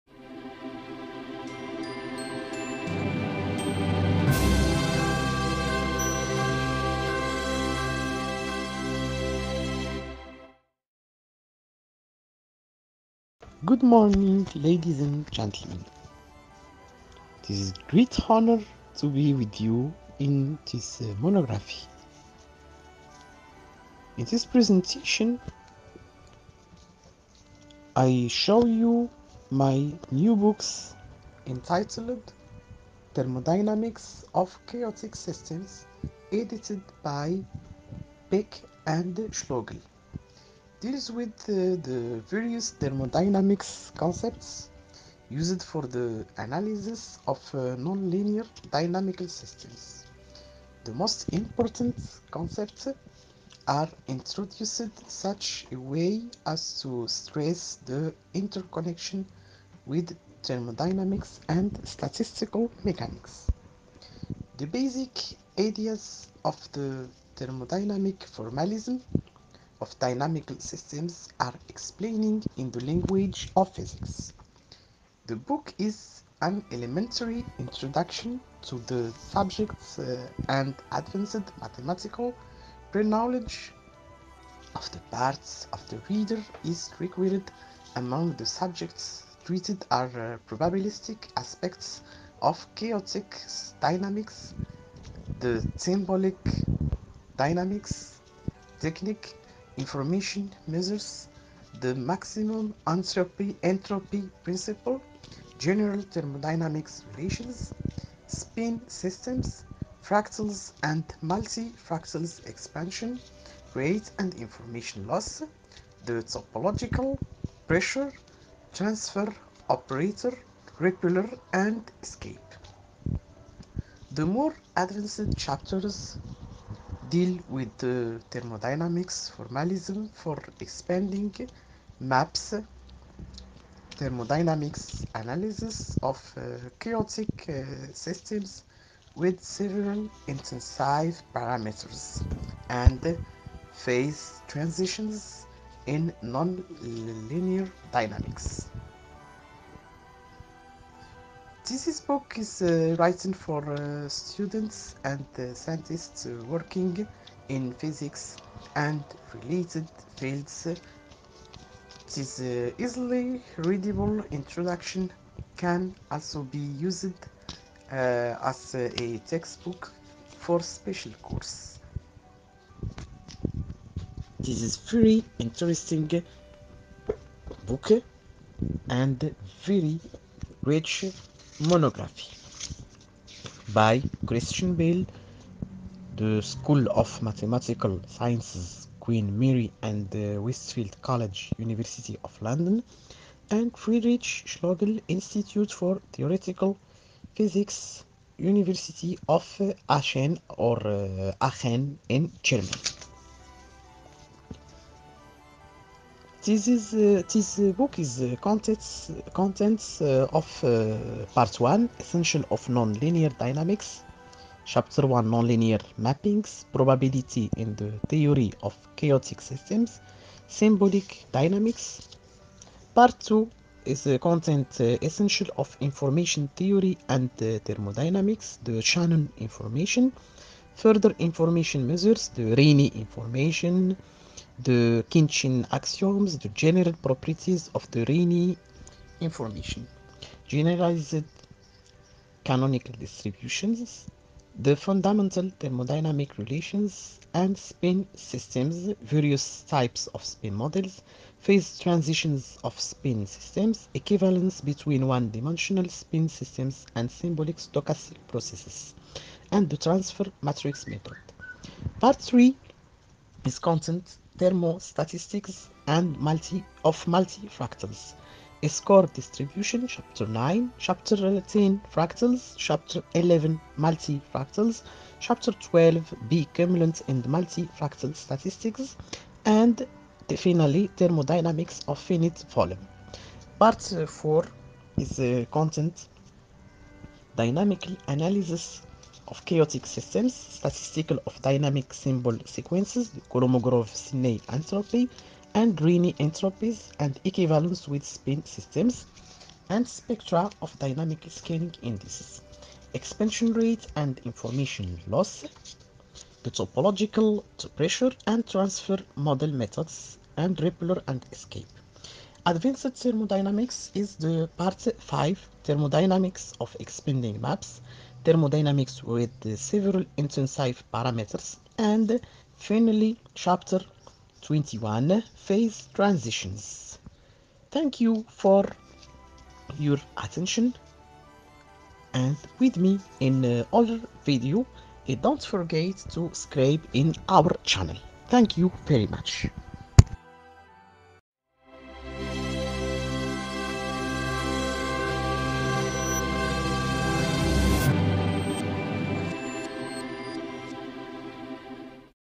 Livre intitulé Thermodynamics of chaotic systems by C.Beck and F.Schlögl
This book entitled «Thermodynamics of chaotic systems» edited  by C.Beck and F.Schlögl  deals with the various thermodynamic concepts used for the analysis of nonlinear dynamical systems .The most important concepts are introduced in such a way as to stress the interconnections with thermodynamics and statistical mechanics .
The basic ideas of the thermodynamic formalism of dynamical systems are explained in the language of physics.The book is an elementary introduction to the subject and advanced mathematical preknowledge on the part of the reader is required.Among the subjects treated are probabilistic aspects of chaotic dynamics,the symbolic dynamics technique,information measures ,the maximum entropy principle,general thermodynamics relations,spin systems,fractals and multifractals,expansion rate and information loss,the topological pressure,transfer operator methods,repellers and escape.The more advanced chapters deal with the thermodynamic formalism for expanding maps,thermodynamic analysis of chaotic systems with several intensive parameters ,and phase transitions in nonlinear dynamics.
Written for students and scientist working in physics and related fields ,this easily readable introduction can also be used as a textbook for special courses.
Review of the hardback edition:
«I enjoyed reading this excellent book .I can easily recommend it. »Journal of Statistical physics.